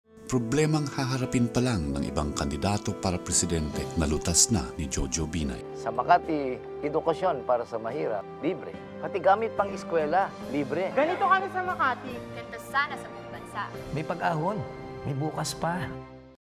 Binay on Education
How do you solve the problem of quality education for Filipinos? Binay answers.